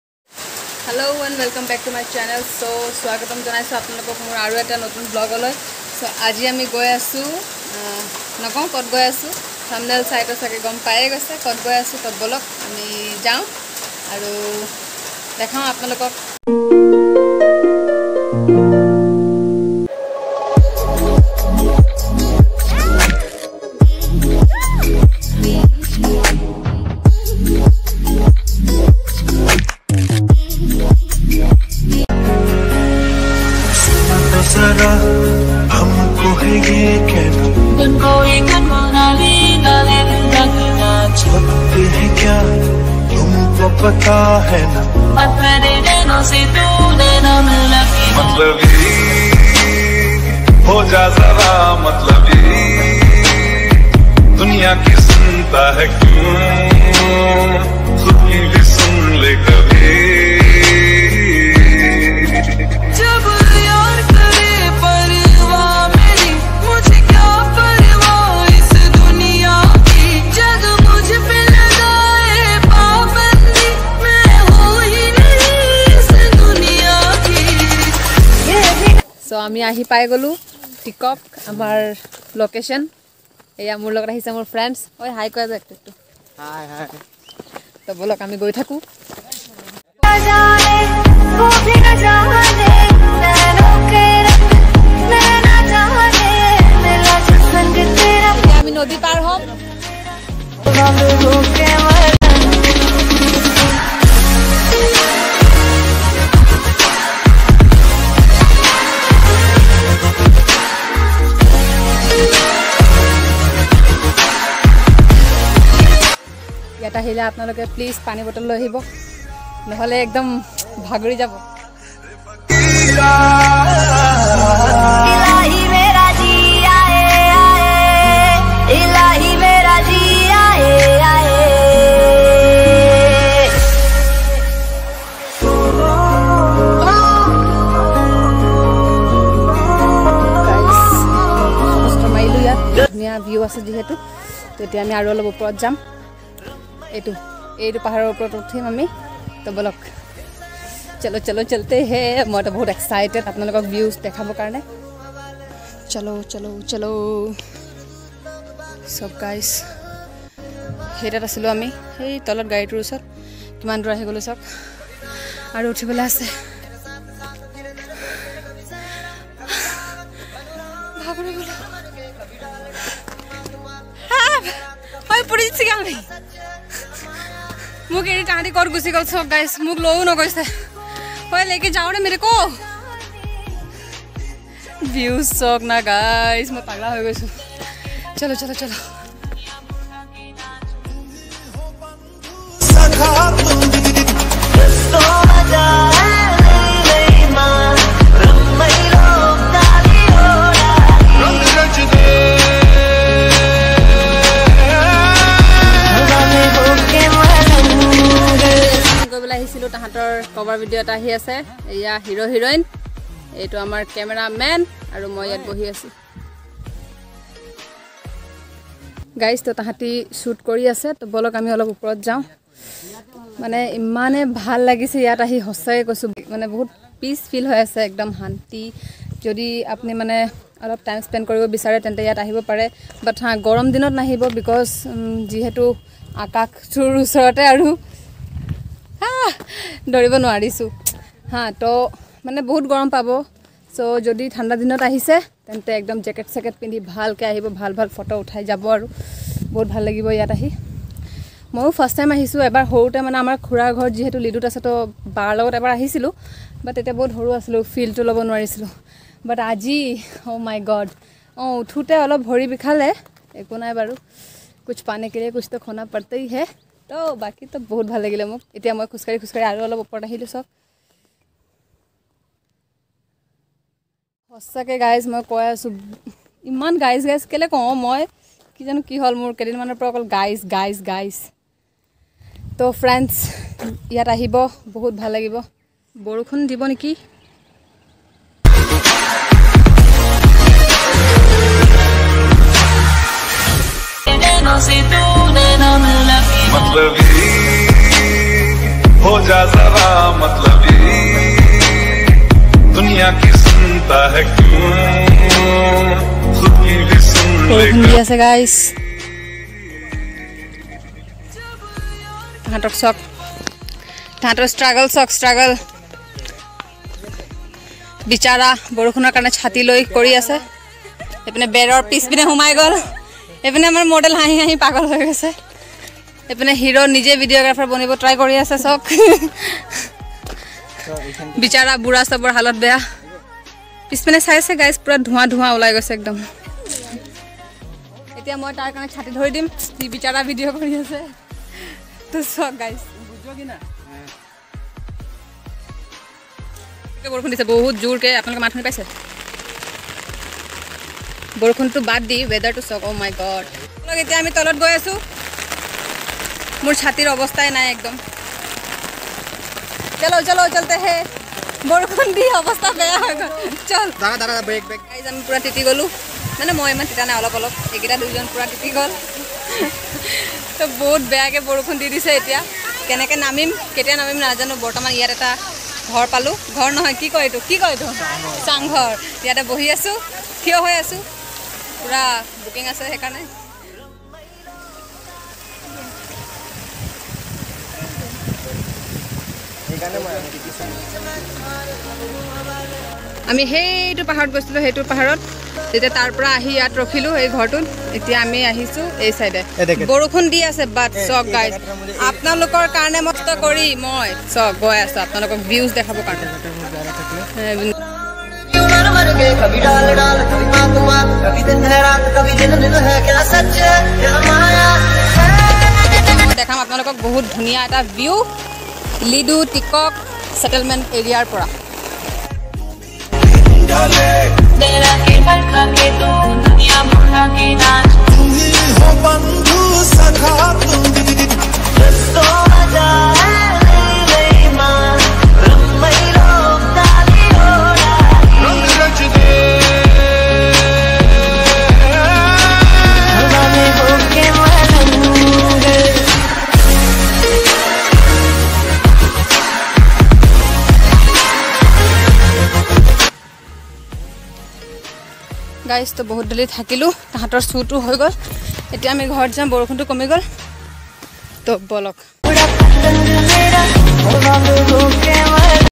LEDO TIKOK COLLIERY||Natural view point Digboi to ledo||Assamese vlog||Rainyday 🌧️Vlog15
Hlo guys ✨
I am back with another video I hope you all will enjoy watching this video❤️✨

Ledo is a small town in Tinsukia district, Assam, India.As of 2005, the Ledo railway station is the easternmost broadgauge railway station in India. The town is also the starting point of Ledo Road, also known as Stilwell Road, a highway built during World War II for use by Americans and British troops as a military supply route to China through Burma (Myanmar).

The first coal mine in Assam was started near Ledo in 1882 when the erstwhile Assam Railway & Trading Company was laying a metre gauge railway line in that region.

Tikak Colliery which is an Open Cast Mine was established in 1986. The mine comes under Makum Coalfield. It is located about 10 Km from Margherita. Tikak Colliery has two Units namely i.e. Tikak East and Tikak West (Tikak OCM).